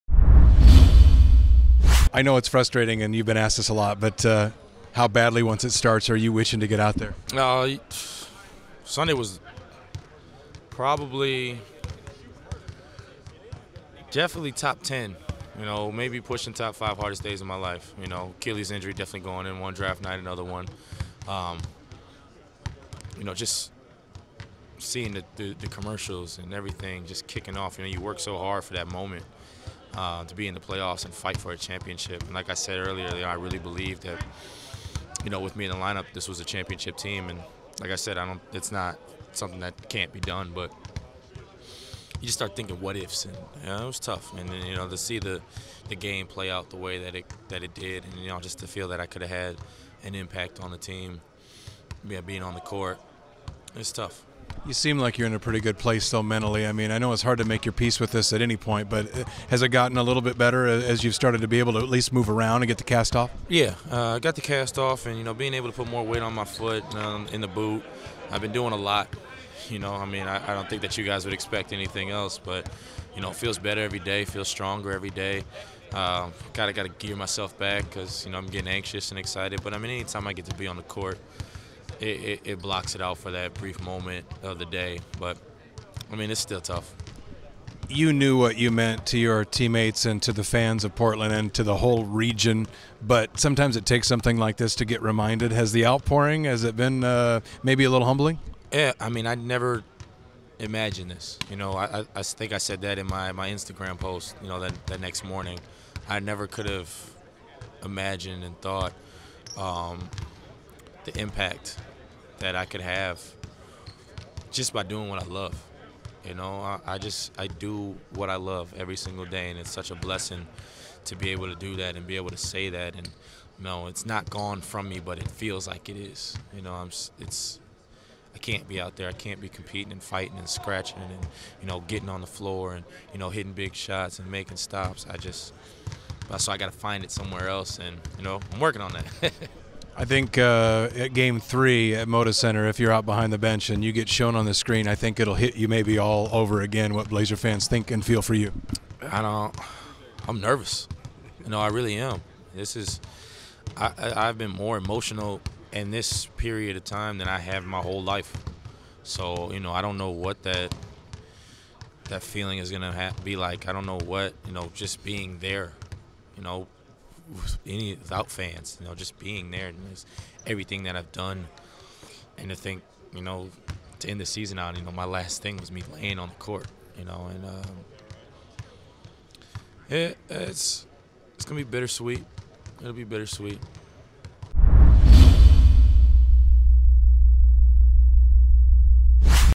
Wesley Matthews Gives Emotional Interview
Wesley Matthews tells Mike Barrett about his struggle while being sidelined for the 2015 NBA Playoffs.